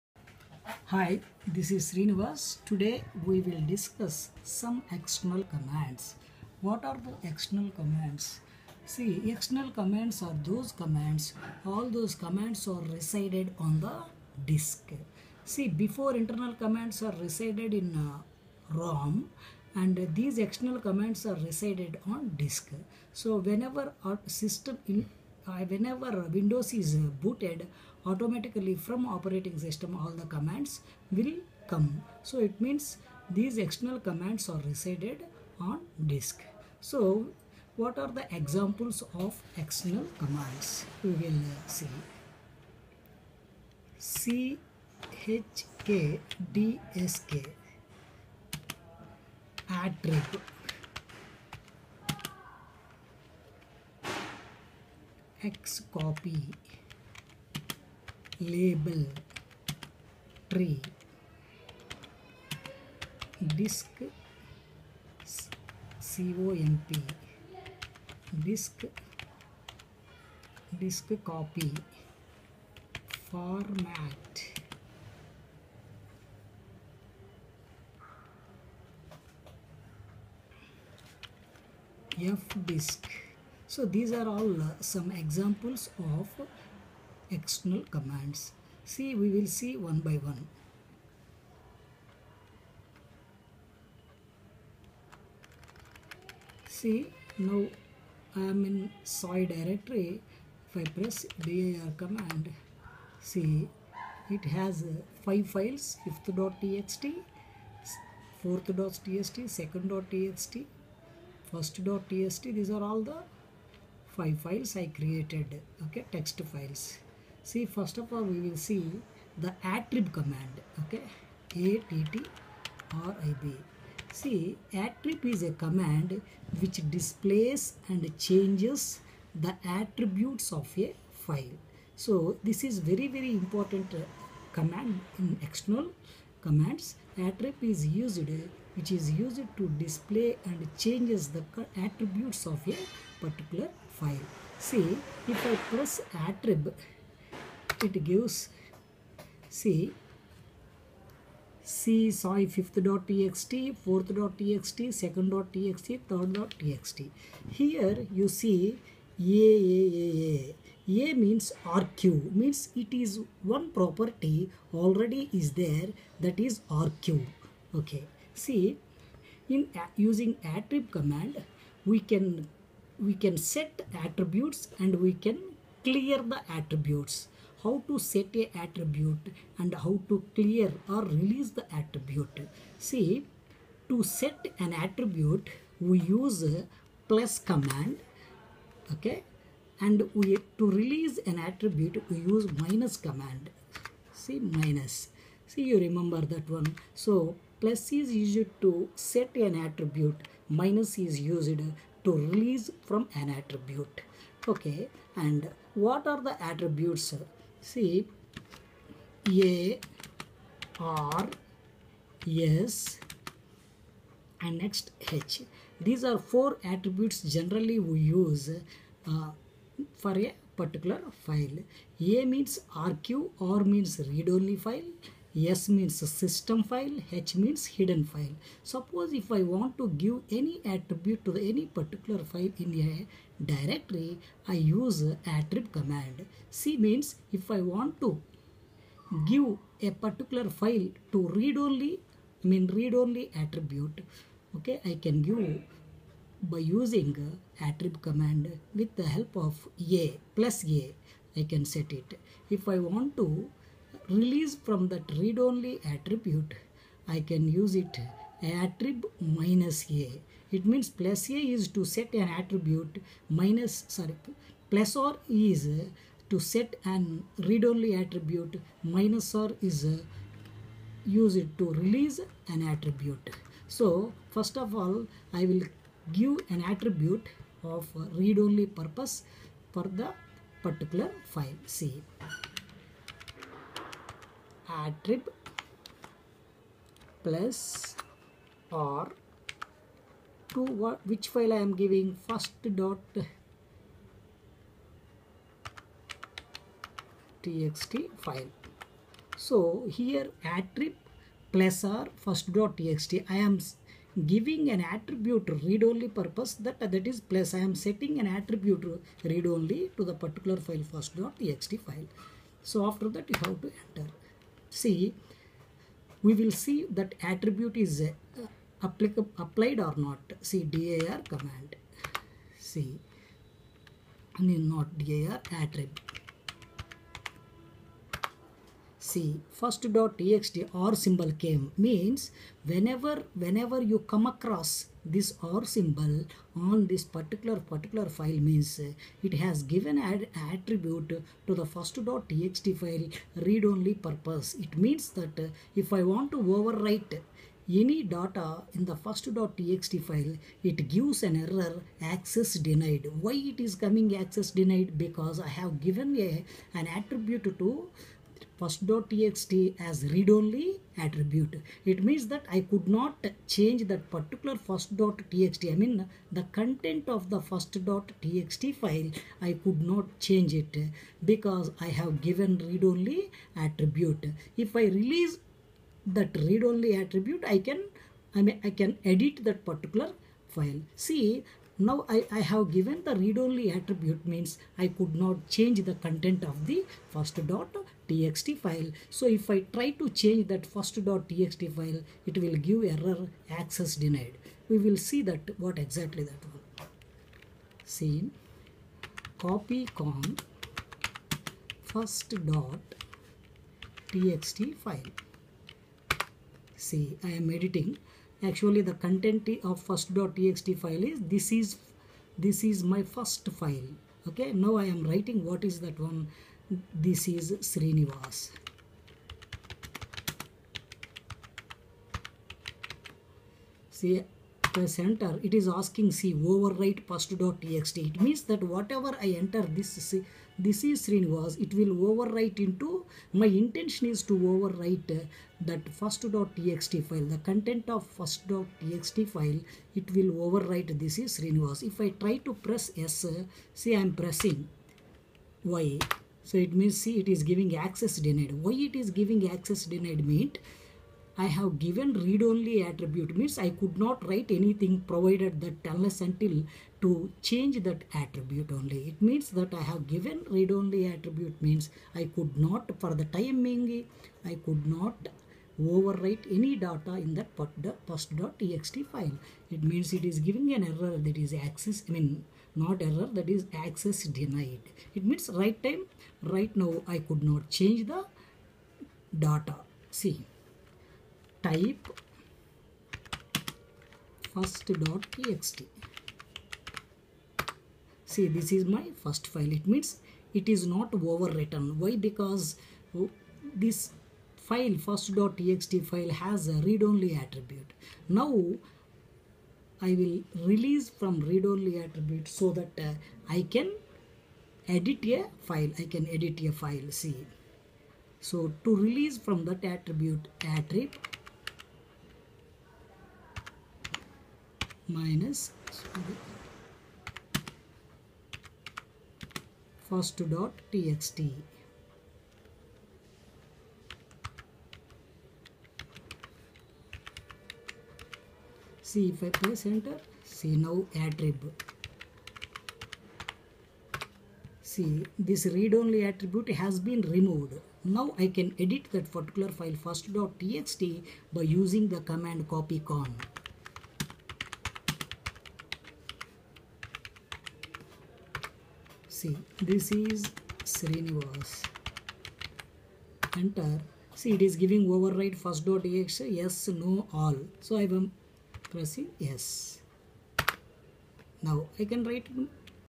dos external commands part4
Dos stands for disk operating system.dos external commands are those commands all the commands are loaded along with operating system.Dos external commands are  CHKDSK, ATTRIB, LABEL,DISKCOPY  etc..